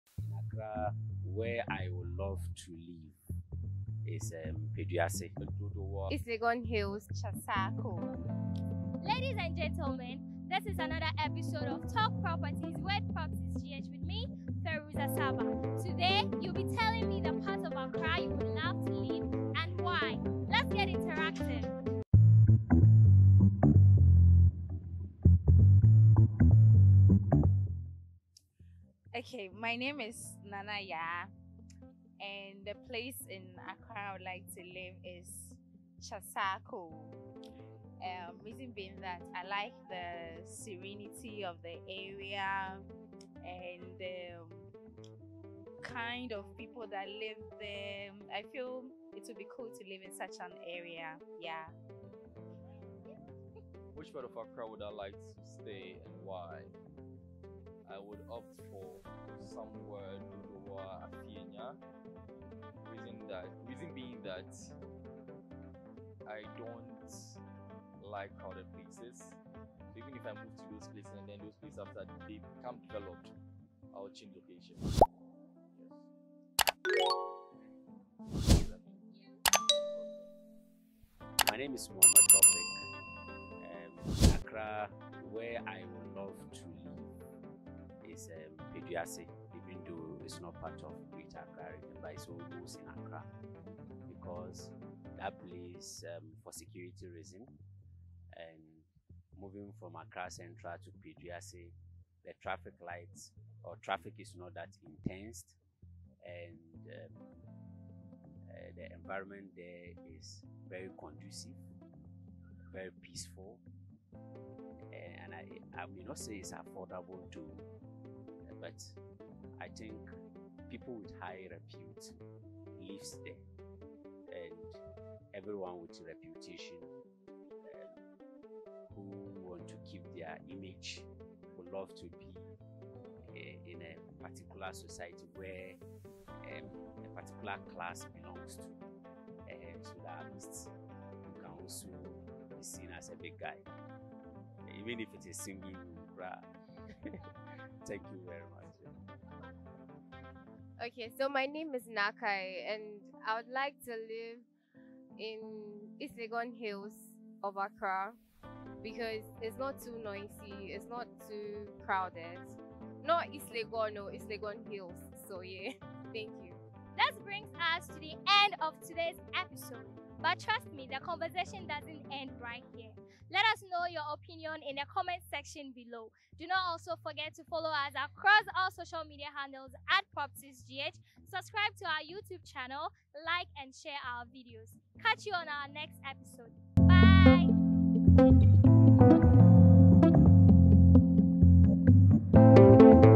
WHERE IN ACCRA WOULD YOU LOVE TO STAY
Explore Accra, Ghana's vibrant capital! 🇬🇭 From stunning beaches to bustling markets, discover the best spots to stay and hidden gems in the city. Join us on this unforgettable journey as we reveal Accra's charm and top accommodations. 🎬🏞️